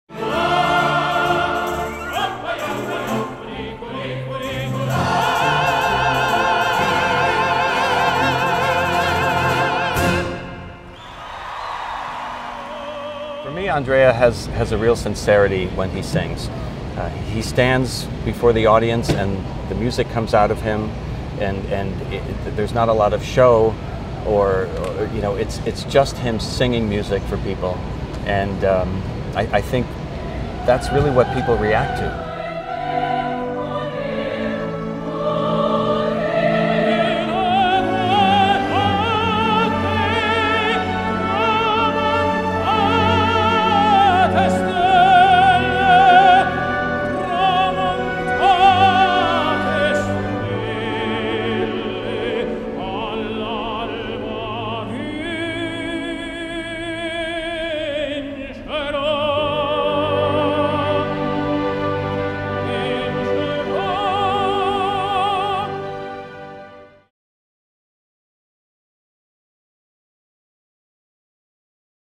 Andrea Bocelli - Central Park (Webisode #11)
Audio Producer David Frost talks about the undeniable sincerity of Andrea Bocelli.